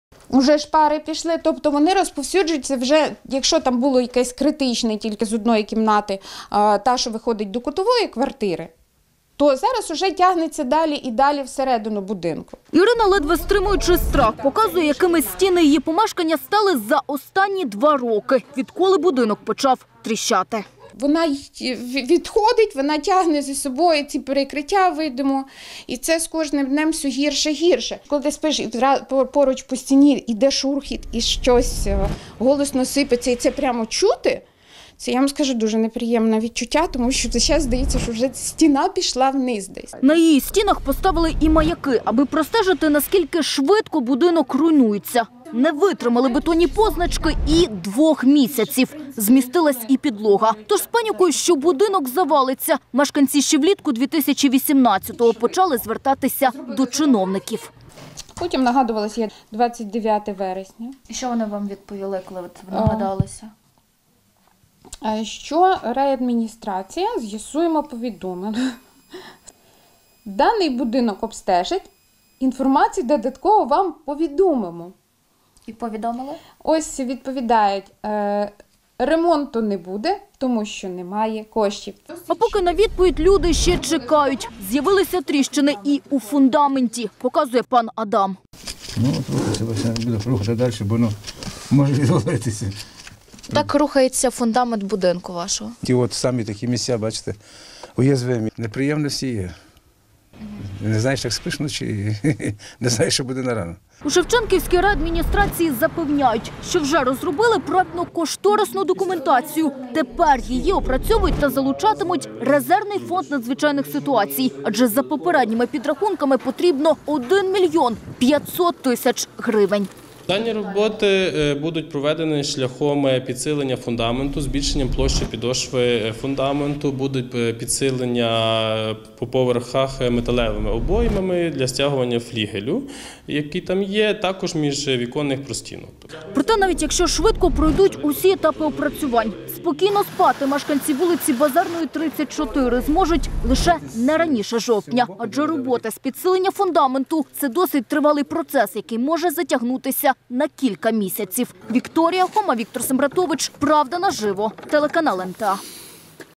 СТІНИ У ТРІЩИНАХ, ЧЕРЕЗ ЯКІ ПРОСТЕЖУЄТЬСЯ СУСІДНЯ КІМНАТА
Ні - це не аварійний будинок, принаймні таким його не вважають, а звичайна старенька триповерхівка у середмісті Львова. Як це - жити в очікуванні, що провалишся  під землю⤵️